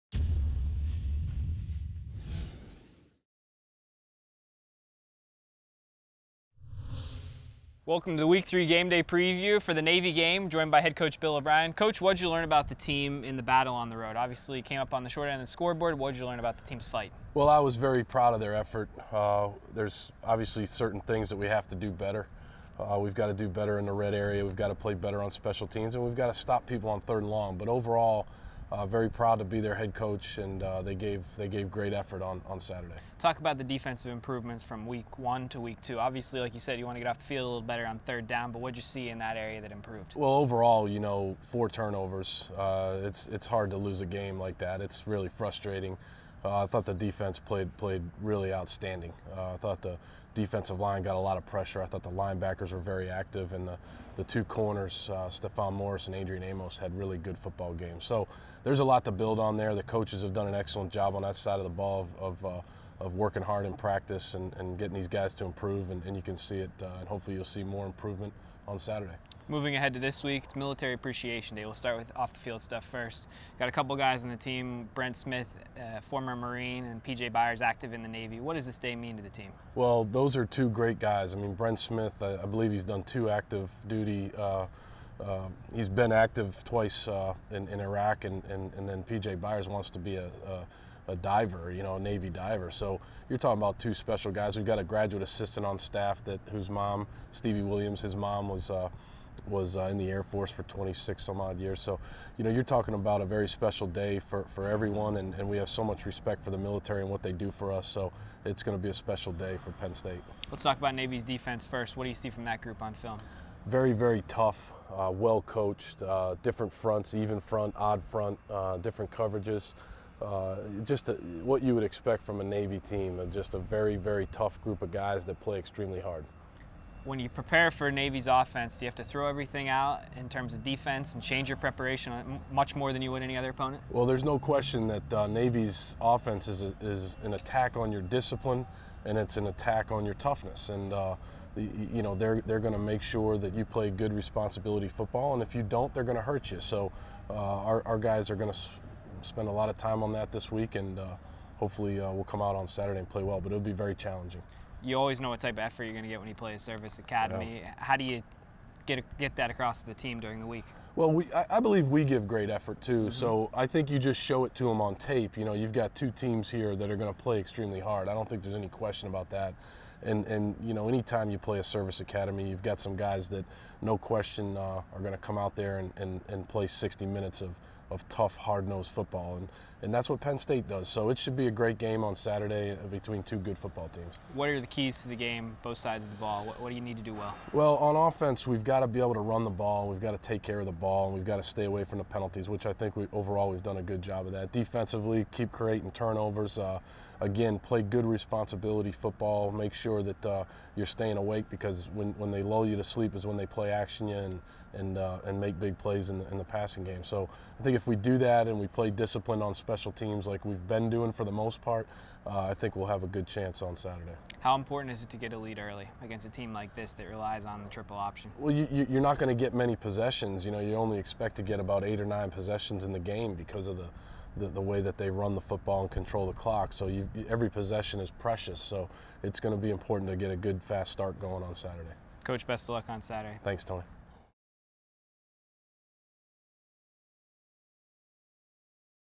Gameday Preview with Coach O'Brien - Navy
Head coach Bill O'Brien previews Penn State's week one matchup against Navy with GoPSUsports.com's Tony Mancuso.